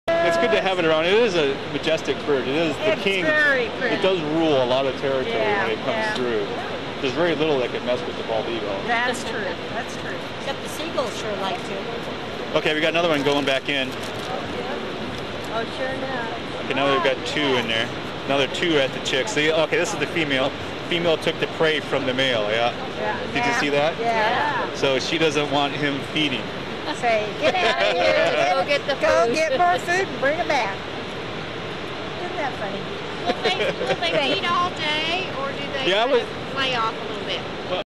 male tries to feed chicks April 25, 2010-Snoqualmie Falls Peregrine Falcons
Male Peregrine Falcon tries to feed the chicks after the female finishes her feeding session. But she flies into the nest a minute later and takes the food from him and caches it for a later feeding. This is a 16:9 format recording.